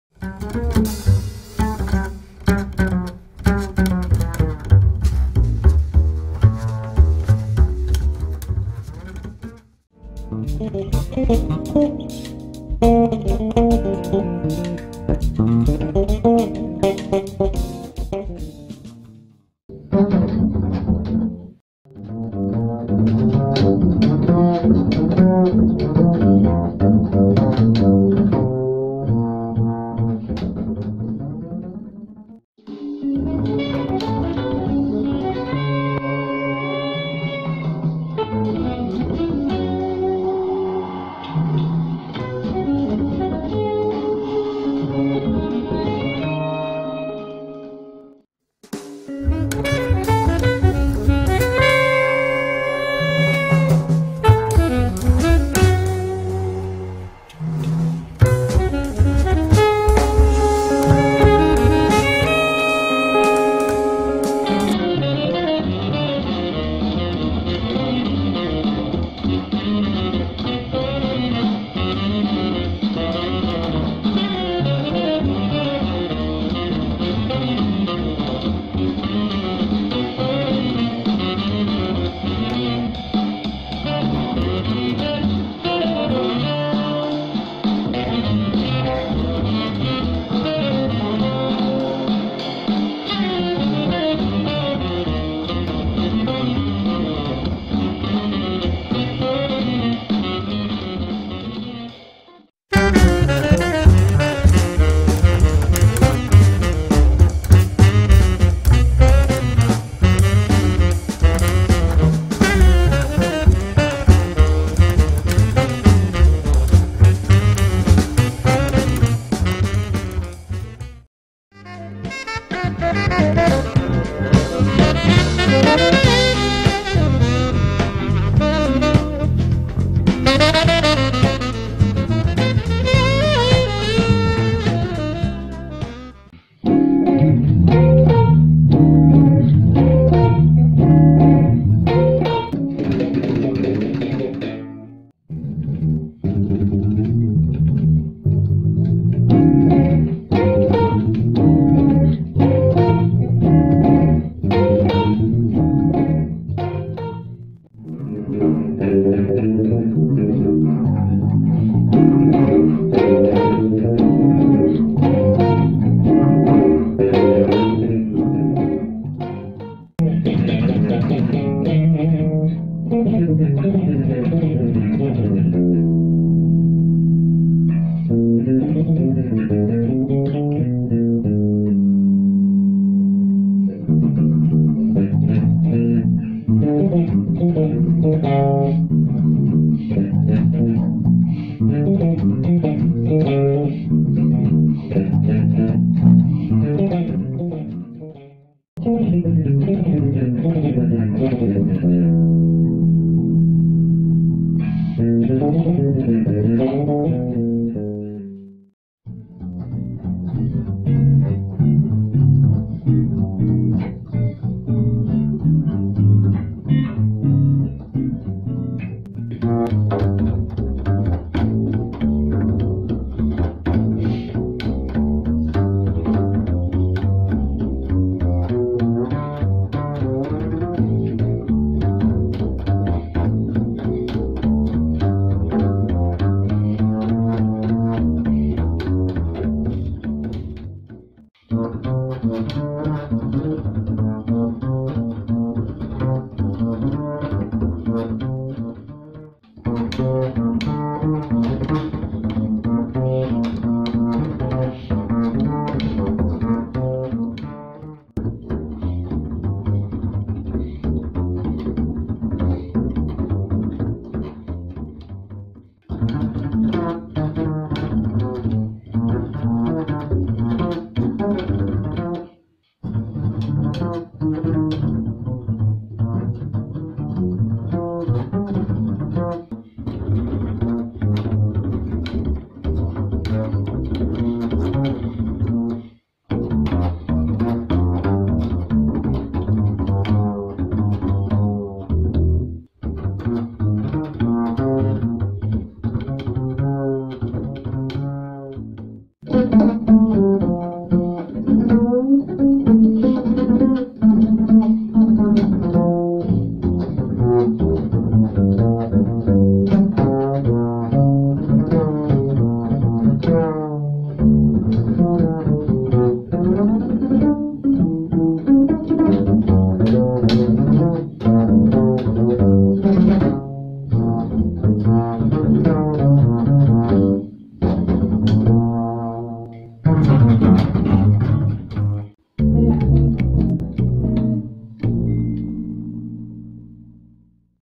Geoff Kluke's "Beneficial Bass Business"
A brief video looking at a few aspects of bass playing. Both Double and Electric bass. I'll also look at a couple of my original compositions plus  fragments of a few well known Bebop tunes.